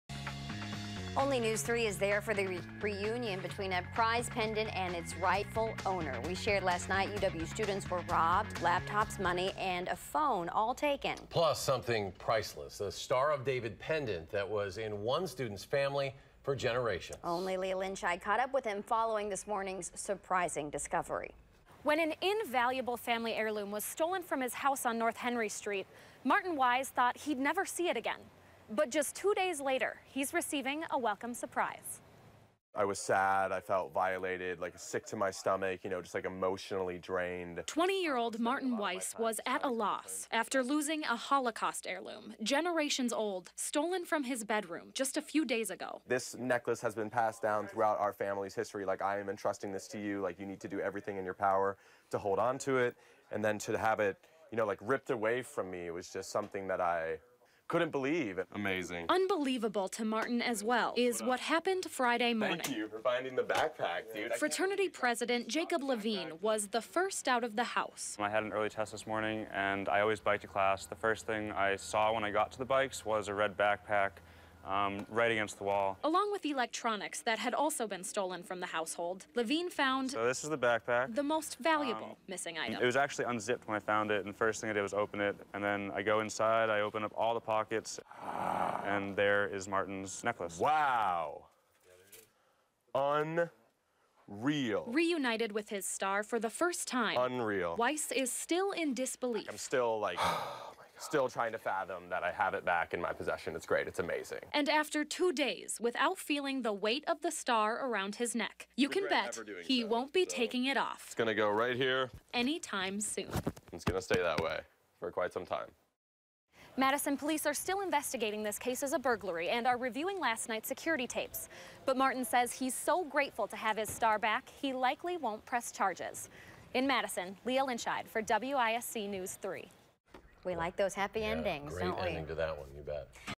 Stolen family heirloom, other items, returned to fraternity house
A theft at a fraternity house on the University of Wisconsin campus left one student without his most prized possession, a family heirloom that survived the Holocaust. But Friday, he received some good news.